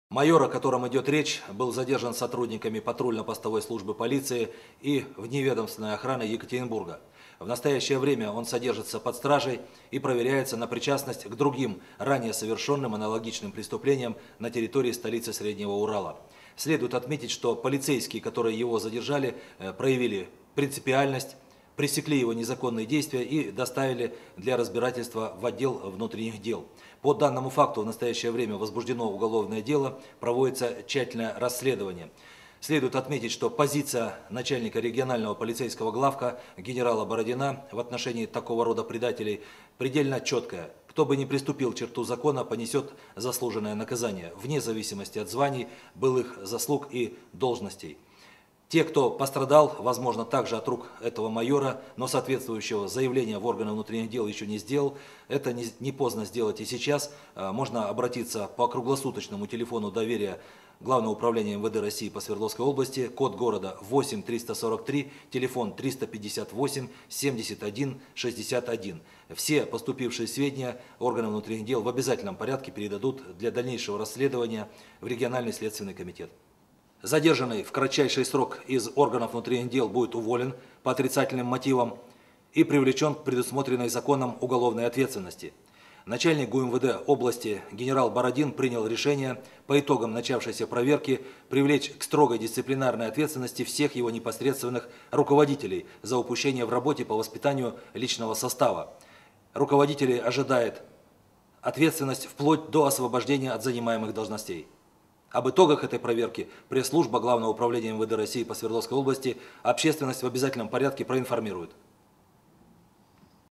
В Екатеринбурге полицейский педофил надругался над 10 летней девочкой
Екатеринбургские полицейские задержали мужчину по подозрению в изнасиловании 10-летней девочки. Каково же было удивление стражей порядка, когда выяснилось, что подозреваемый — их коллега в чине майора.